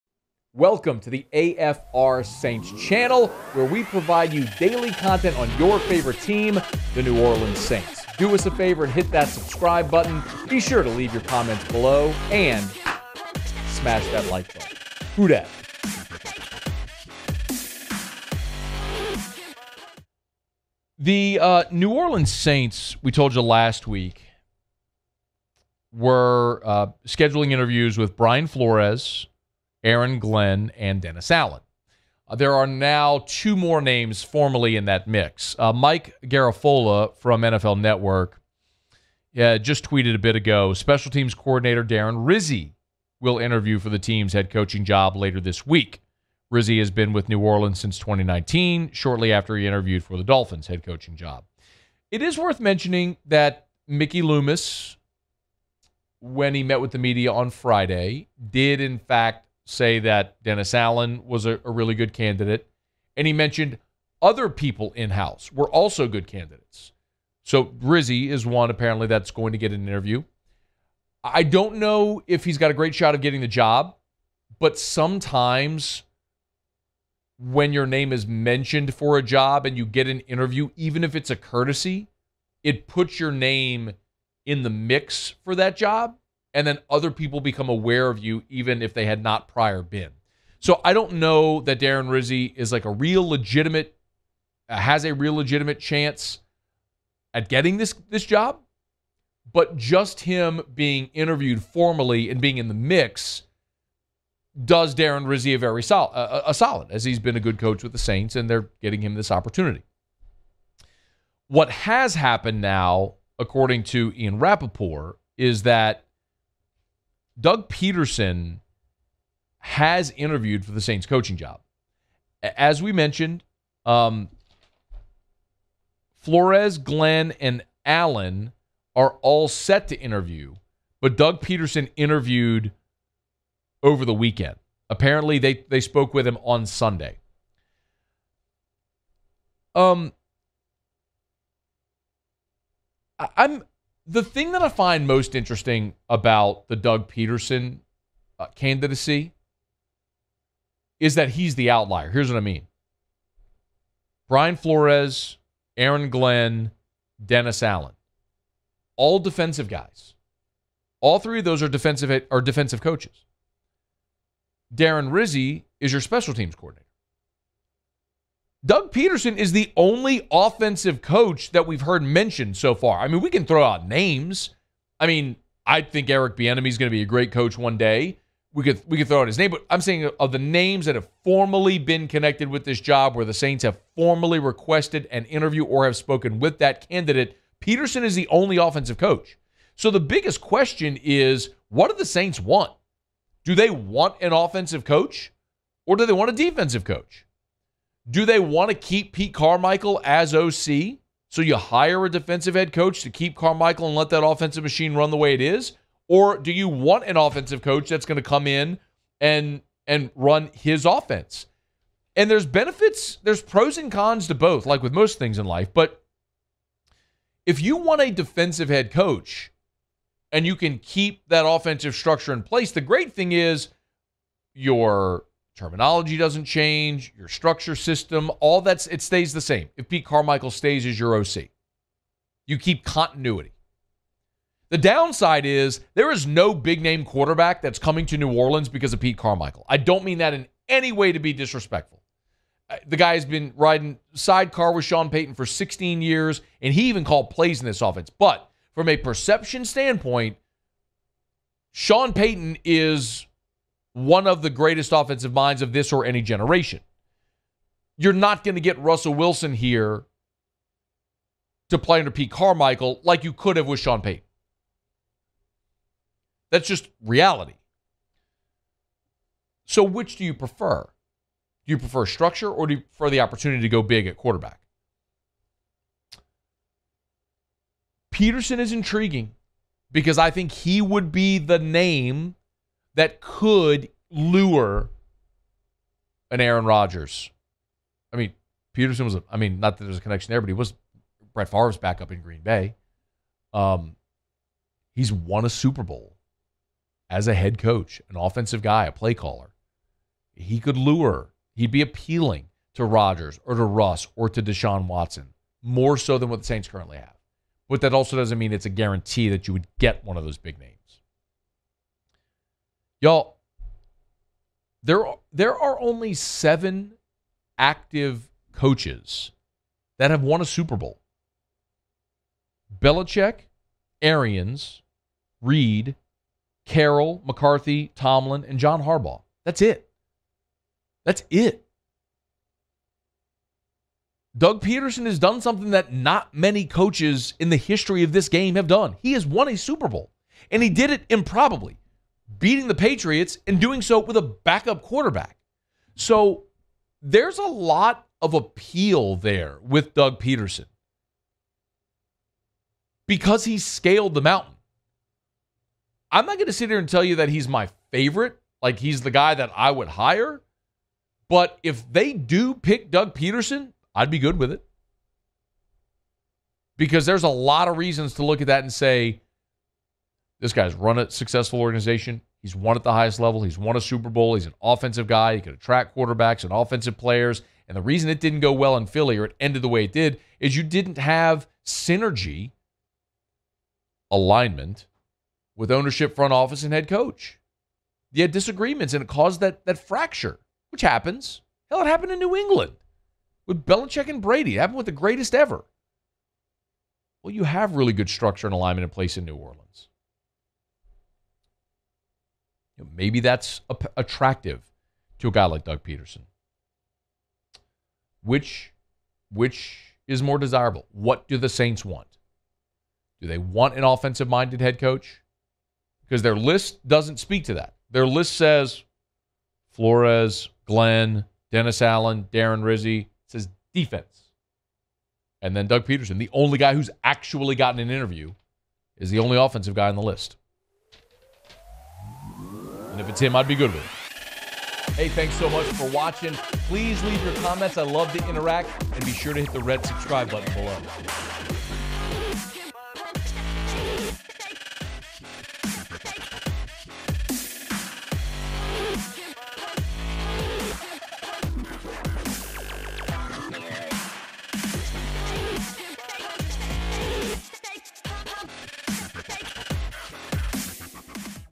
Are the Saints leaning toward hiring Doug Pederson?
The Saints have started the interview process to replace Sean Payton as head coach. Former Eagles head coach Doug Pederson has interviewed with the Saints. Matt Moscona reacts to the news and explains why he would be okay if Pederson were to be hired.